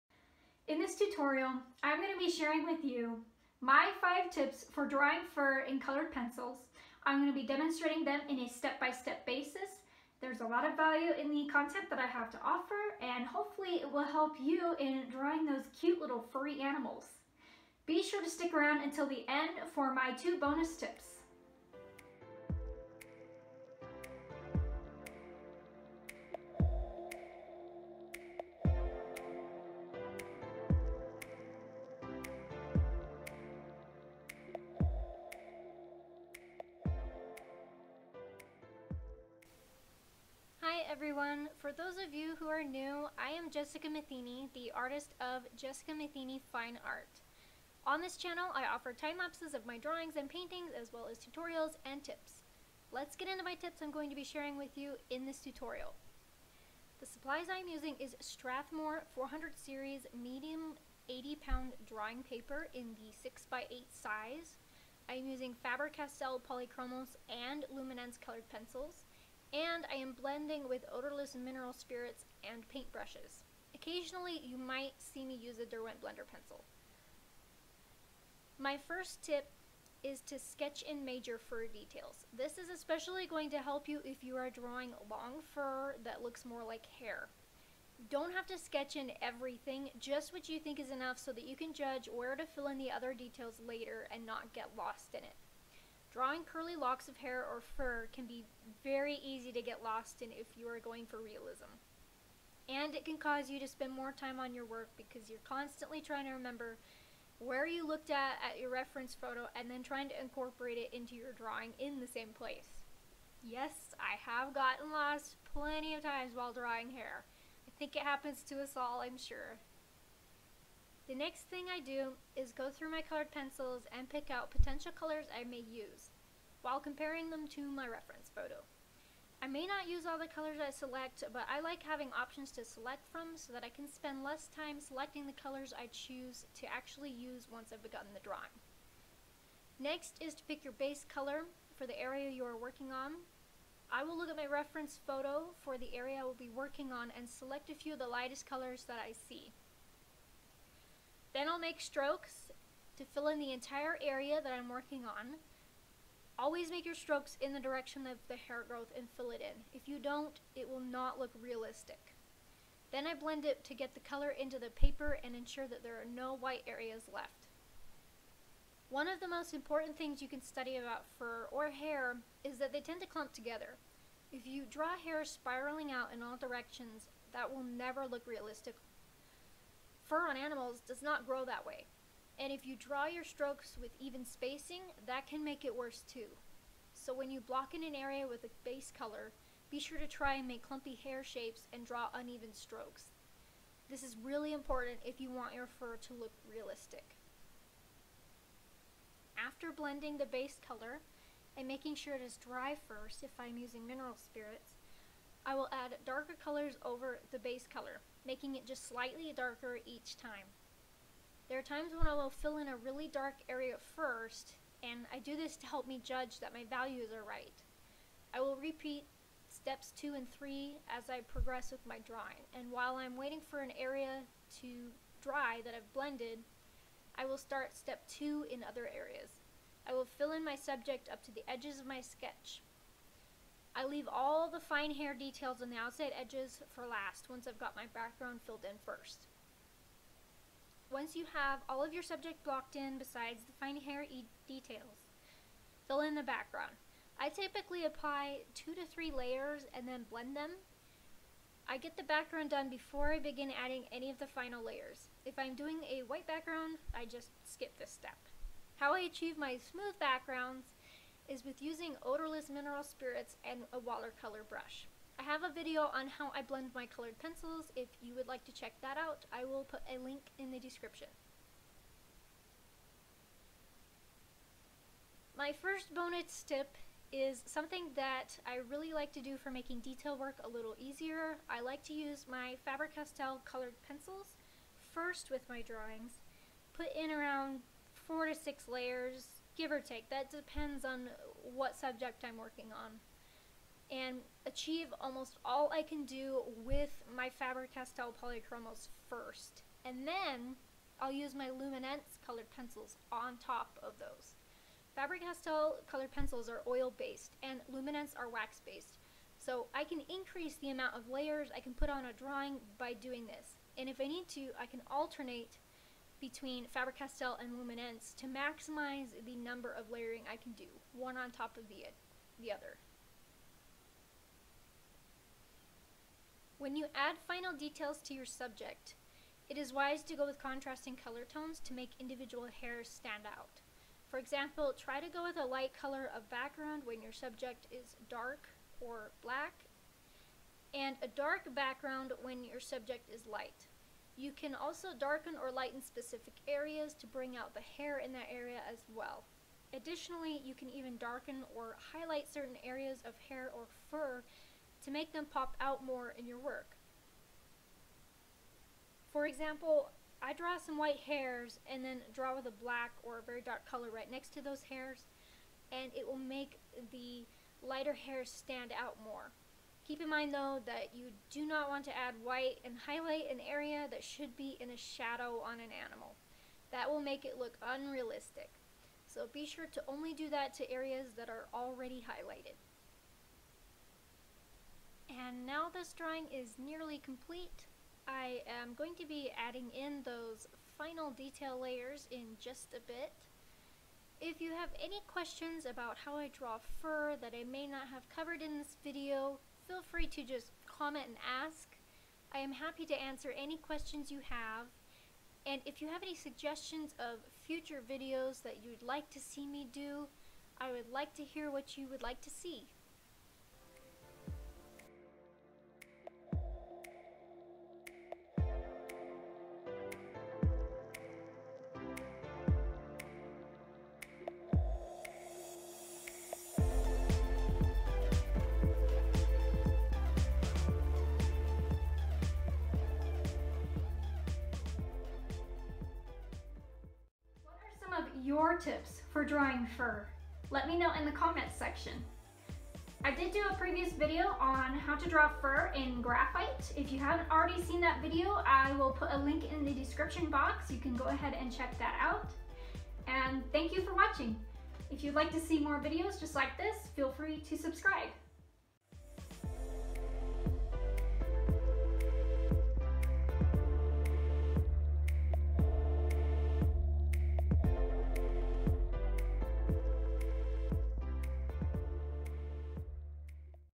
Drawing Fur With Colored Pencils
In this video I talk about my 5 tips for drawing fur in a step-by step process while I demonstrate drawing a Cocker Spaniel.

- Faber Castell Polychromos 60
- Strathmore Bristol Vellum 9x12” Pad
- Sandpaper Sharpener
- Faber-Castell 9000 Graphite Pencils
- Tombow Mono Zero Eraser
- Faber-Castell Pencil Eraser & Brush
- Mona Lisa Odorless Mineral Spirits
- Caran Dache Luminance White Colored Pencil
- Caran Dache Luminance Payne’s Grey 30%
- Caran Dache Luminance Steel grey
- Round Gold Taklon Paint Brushes (#3 & #7)
- Round Synthetic Squirrel Watercolor Brush (#8)
- Kum Sharpener
- Glassine Roll (used under my hand)